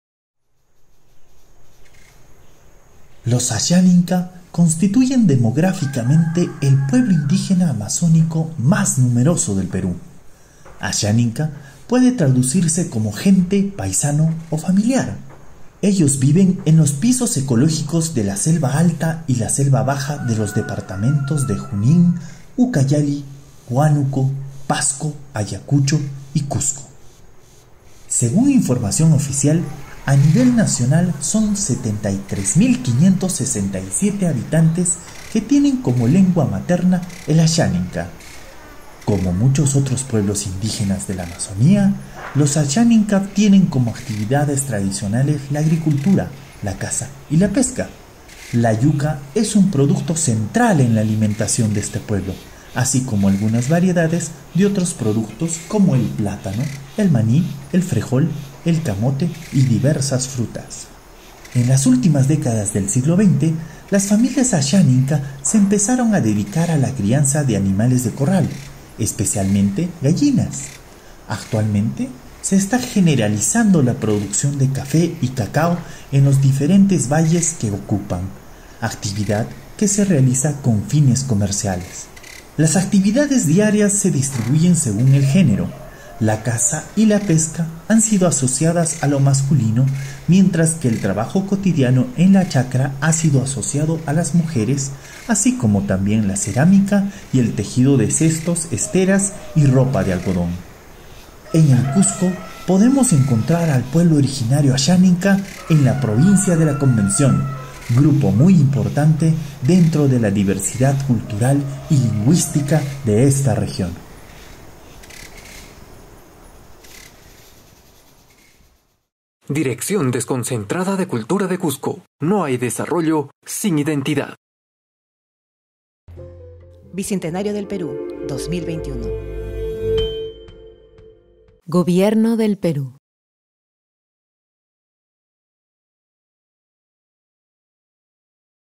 PUEBLO ORIGINARIO ASHANINKA
La Dirección Desconcentrada de Cultura, elabora vídeos para difundir la diversidad cultural de nuestra región.
¿Sabias que en el Cusco existen 8 Pueblos Originarios? Aquí de brindamos información sobre el Pueblo Originario Ashaninka.